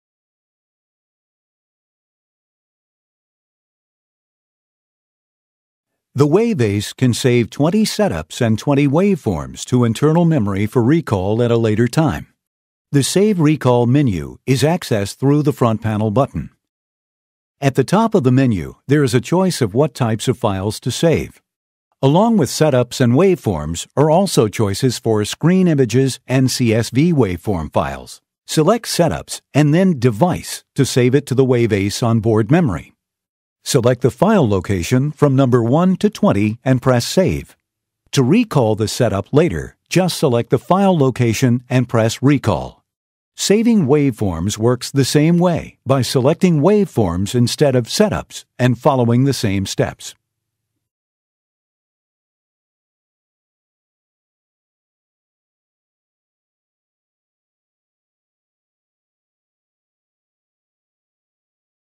LeCroy WaveAce Oscilloscope Series - Saving Waveforms And Setups
WaveAce has USB port on the front panel for saving waveforms, setups and screen shots to a memory stick

Rear panel USB connects to printers for hardcopy screen shots

Rear panel also provides PC connectivity through EasyScope software for saving waveforms, setups and screenshots via USB and LAN

The LeCroy WaveAce combines long memory, a color display, extensive measurement capabilities, advanced triggering and excellent connectivity to improve troubleshooting and shorten debug time. With bandwidths from 60 MHz to 300 MHz, sample rates up to 2 GS/s and waveform memory up to 9 kpts/Ch (18 kpts interleaved) the WaveAce exceeds all expectations of a small affordable oscilloscope.

Video By LeCroy.